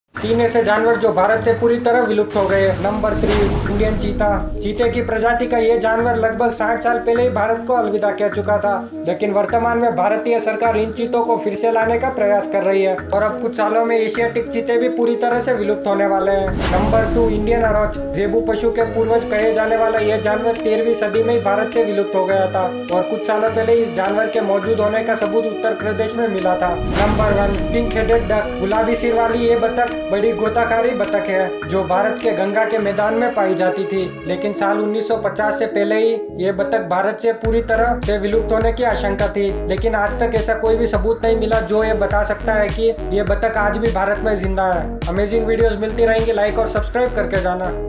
ये जानवर 🇮🇳 से खत्म होगाए 😨 | animals that are extinct from 🇮🇳
Animals that are extinct from India
animals that are extinct from the world

credit: effective video intros